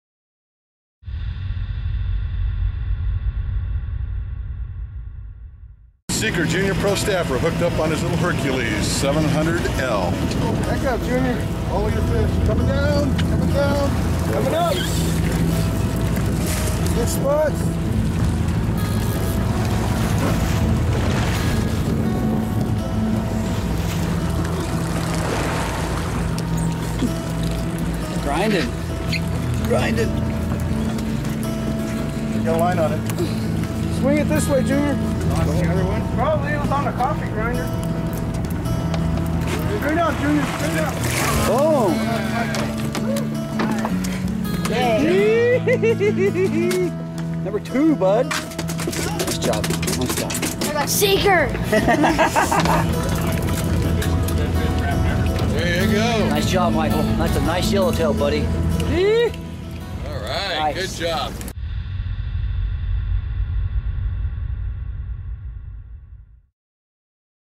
JR PRO STAFF.mov
Pro Staffer Michael Luther puts the hurt to a big yellowtail on the Top Gun 80